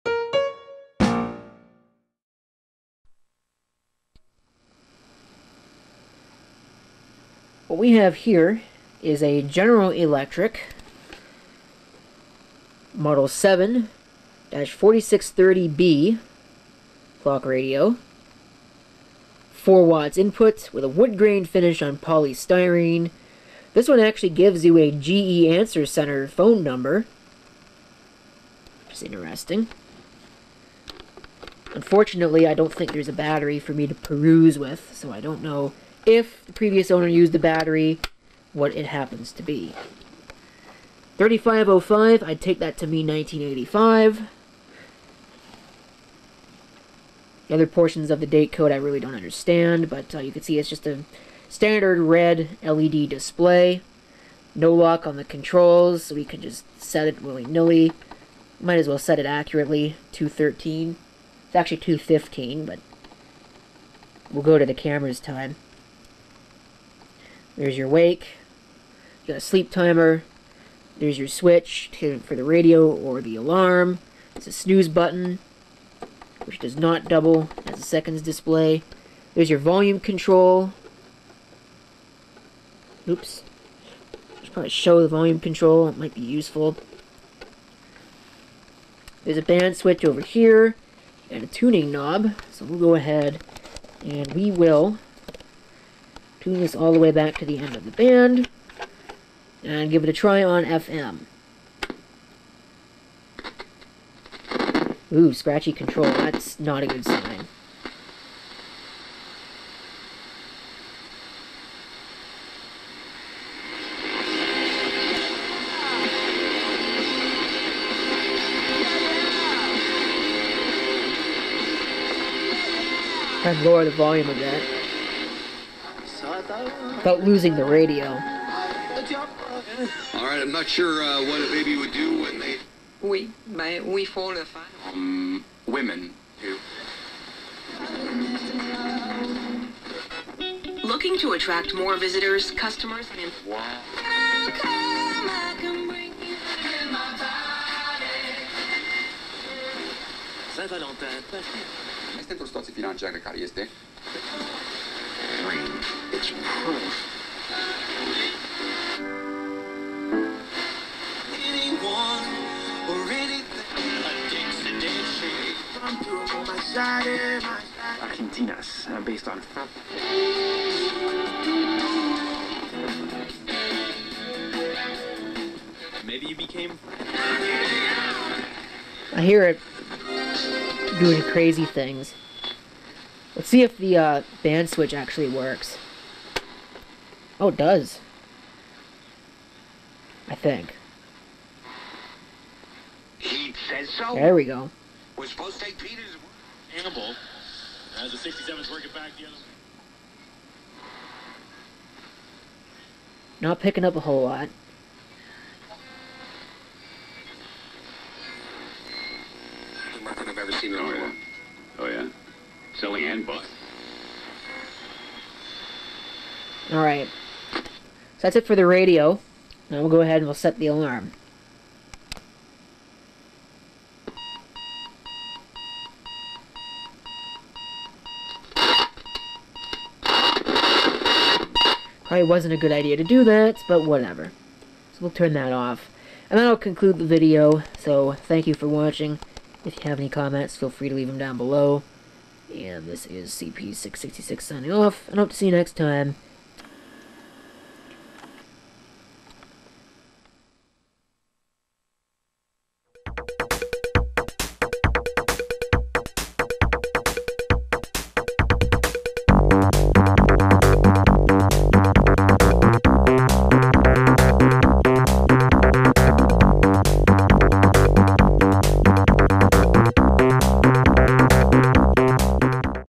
General Electric 7-4630B Clock Radio
Not a whole lot to write home about, but I had to buy something to bring the value of a purchase up for the purposes of fulfilling an exchange, so it followed me home.

Includes an unintentional "sneak peek" at a future clock radio after I inadvertently recorded over top of the fade. Oops. :-P